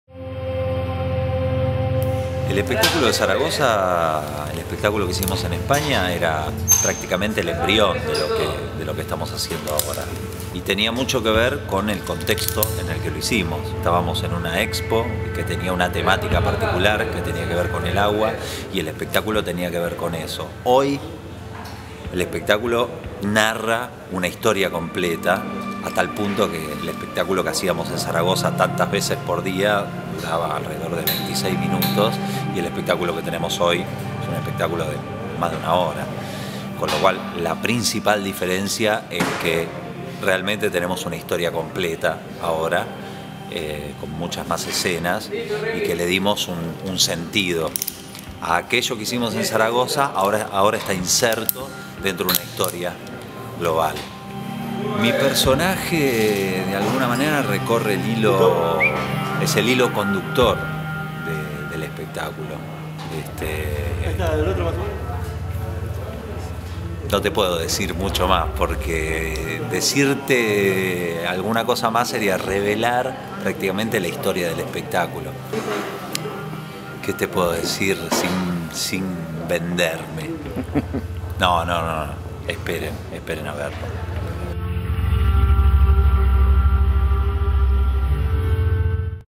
Entrevista Leo Kreimer - 1a Parte
-- Leonardo Kreimer, protagonista y asistente de dirección de Hombre Vertiente, cuenta las diferencias entre el show de Zaragoza de 2008 y la nueva presentación en Buenos Aires desde septiembre 2011 en el Centro Cultural Recoleta.